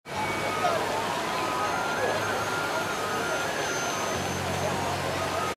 Boat at Chinese Lantern Festival at Texas State Fair 2013
The boat at the Chinese Lantern Festival at the State Fair of Texas 2013